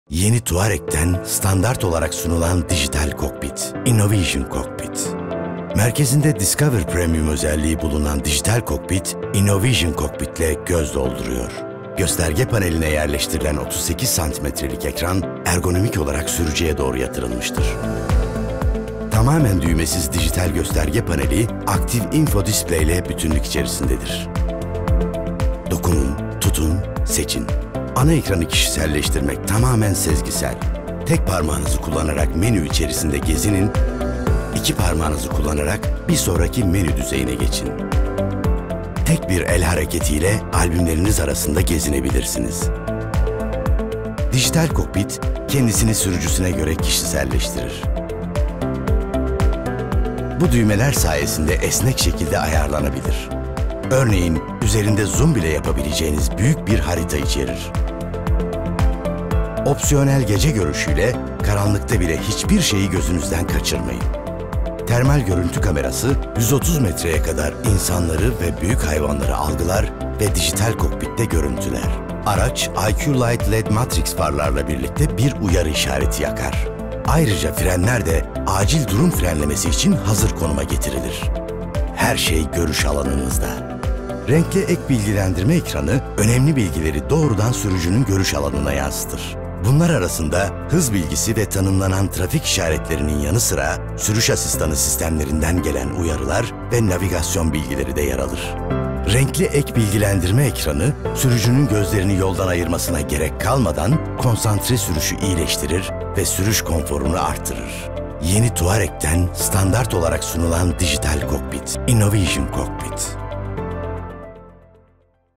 Yeni Touareg Teknolojileri – Dijital Kokpit “Innovision Cockpit.”
Yeni Touareg, yeni standartlar yaratıyor. Merkezinde Discover Premium radyo navigasyon sistemi bulunan Dijital Kokpit “Innovision Cockpit”’te, gösterge paneline güvenli bir şekilde yerleştirilen 38 cm’lik ekran bulunur. Üzerinde tuş veya düğme bulunmayan ekranda menüler arasında geçiş tamamen sezgiseldir.
Tek parmağınızı kullanarak menü içerisinde gezinebilir, iki parmağınızı kullanarak bir sonraki menü düzeyine geçebilirsiniz. Üstelik “Night Vission” Gece Görüşü ile karanlıkta bile hiçbir şeyi gözünüzden kaçırmazsınız. Termal görüntü kamerası, 130 metre mesafeye kadar insanları ve büyük hayvanları algılayarak Dijital Kokpit “Innovision Cockpit”e yansıtır.
Renkli Ek Bilgilendirme Ekranı, önemli bilgileri doğrudan sürücünün görüş alanınıza yansıtır. Bunlar arasında, hız bilgisi ve tanımlanan trafik işaretlerinin yanı sıra sürüş asistanı sistemlerinden gelen uyarılar ve navigasyon bilgileri de yer alır. Renkli ek bilgilendirme ekranı, gözlerinizi yoldan ayırmanıza gerek kalmadan konsantre sürüşü iyileştirir ve sürüş konforunuzu artırır.
Yeni Touareg’den Dijital Kokpit “Innovision Cockpit.”